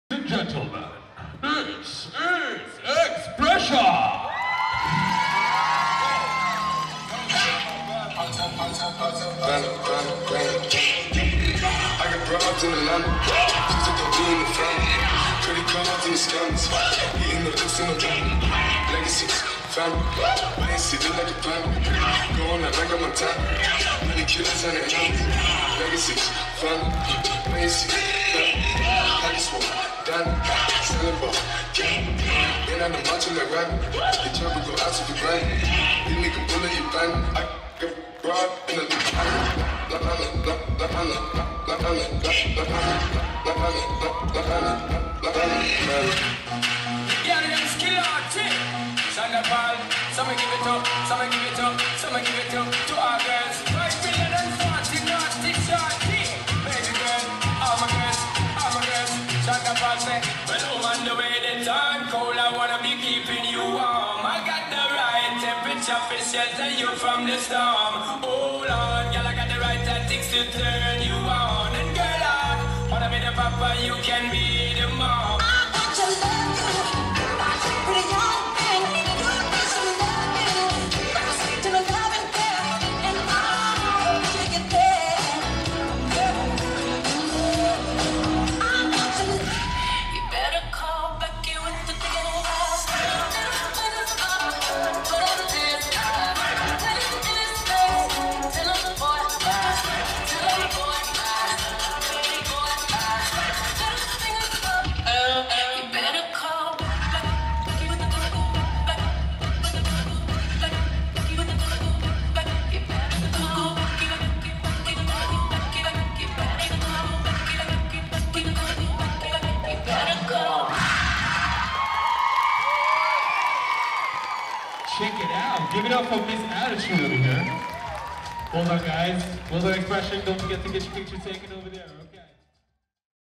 X PRESSION! - BDO South West Street Dance Championships 2016
Team performance from the BDO South West Street Dance Championships - Olympia Centre Chippenham December 2016